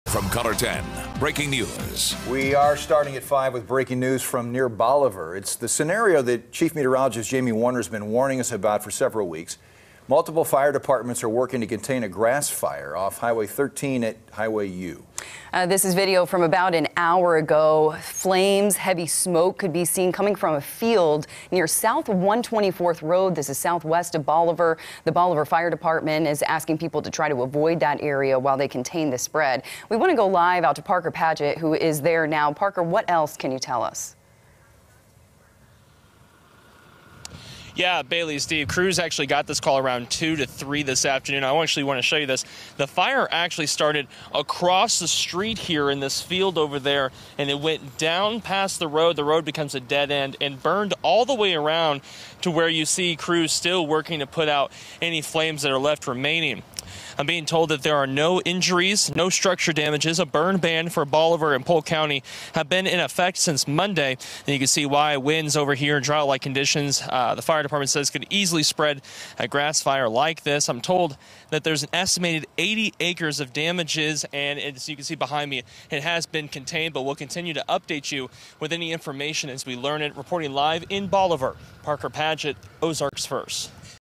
80 acres damaged in grass fire near Bolivar Wednesday afternoon
A burn ban is in effect for Polk County and the City of Bolivar, as multiple crews fought a grass fire Wednesday afternoon. Parker Padgett was live on the scene to show how large the fire was.